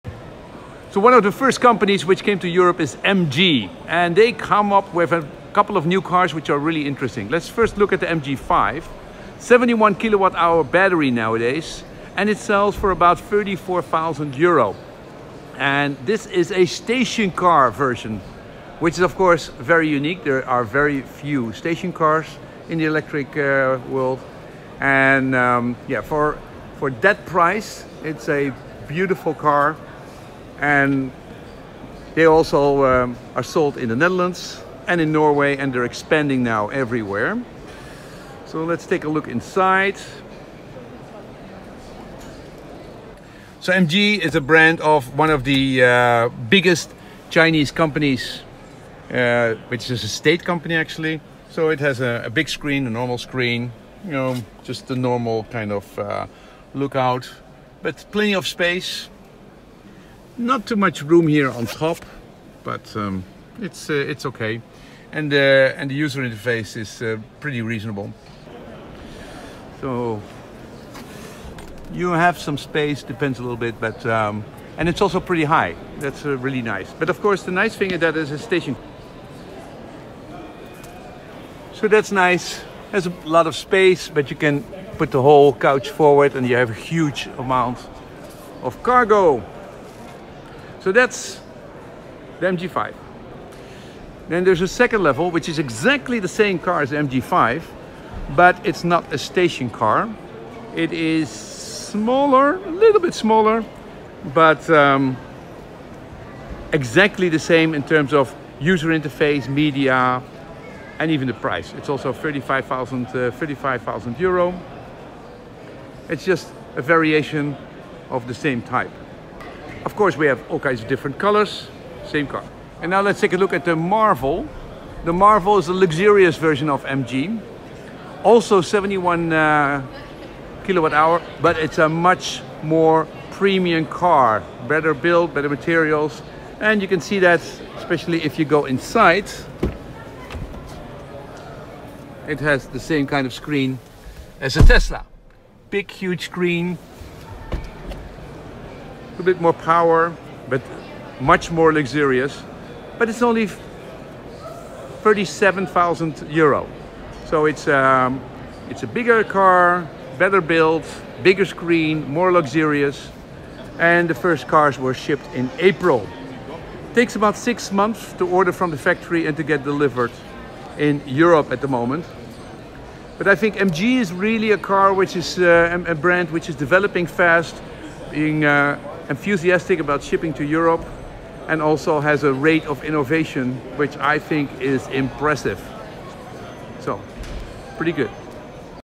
MG update 72kwh battery, 400km range, stationcar MG5 35k Marvel €40k
Tour of the MG product line. Great cars from China from €34k with 400 range. They can deliver and are of good quality. 2nd largest car company of china with 7 million production a year

Live updates from evs35 evs35nl daily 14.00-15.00